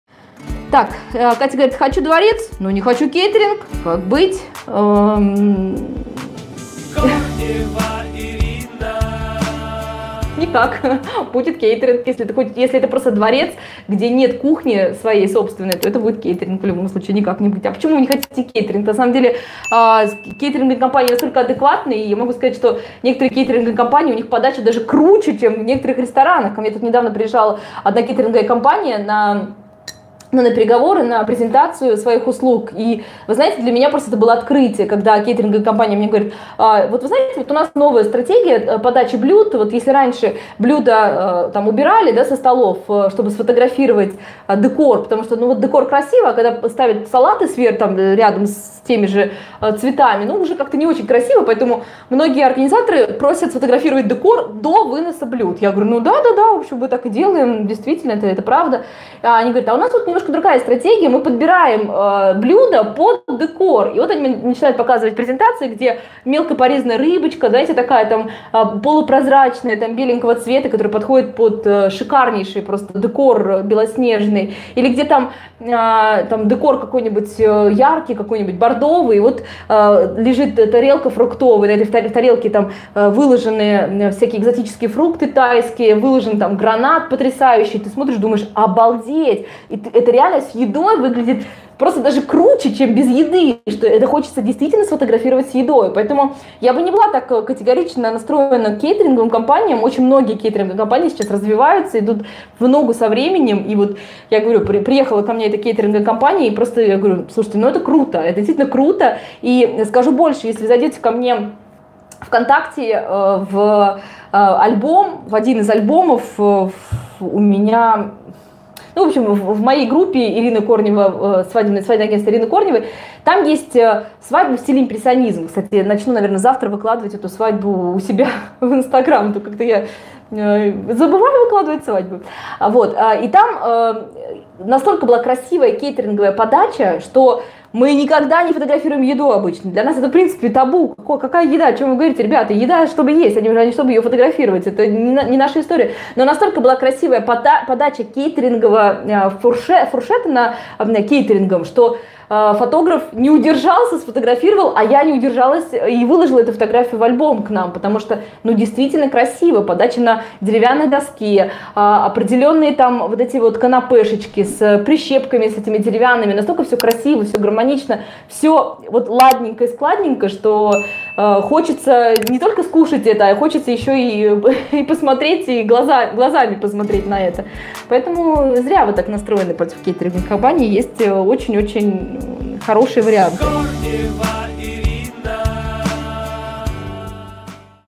Можно ли обойтись без кейтеринга на свадьбе? Ресторан или кейтеринг, что выбрать?
Привет, мои невесты!
Есть банкетные площадки, в которых нет ресторана. Это просто помещение, которое не подразумевает ни кухни, ни обслуживания. Можно ли обойтись без кейтеринга на свадьбе, где нет собственной кухни.
Как ни крути, но никак не обойдёшься без кейтеринга в таких случаях.
Ресторан или кейтеринг что выбрать? Есть ли плюсы у кейтеринга перед рестораном? Есть, при определённых условиях. Ресторан это стационарная кухня, порой имеет гораздо большие мощности.
У кейтеринга кухня отличается, и тут каждый выбирает под формат мероприятия. Вам в любом случае, будь-то кейтеринг, или ресторан, следует пробовать дегустационный набор.
Иначе вы никак не поймёте вкусная еда или нет.
Сейчас кейтеринг на свадьбе, это не походная еда, не полевая еда, это не еда для пикника, это полноценное меню, причём часто это интересная подача блюд.
Кейтеринговым компаниям приходится конкурировать друг с другом, и подача блюд это один из пунктов конкуренции.
Ресторан конкурирует обычно помещением, кухня идёт вместе с помещением, поэтому у ресторана нет конкуренции, кто будет обслуживать банкет, у них проще.